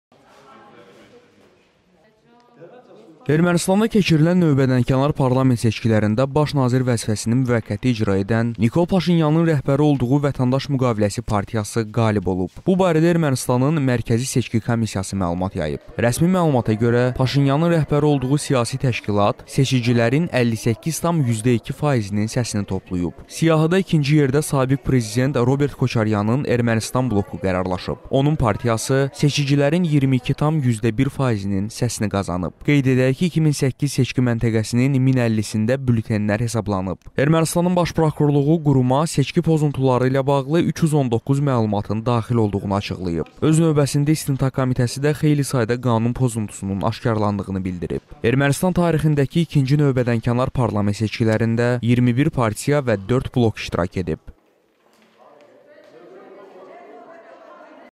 Paşinyanın partiyası qalib olub
"Vətəndaş müqaviləsi" partiyası seçicilərin 58,02 %-nin səsini toplayıb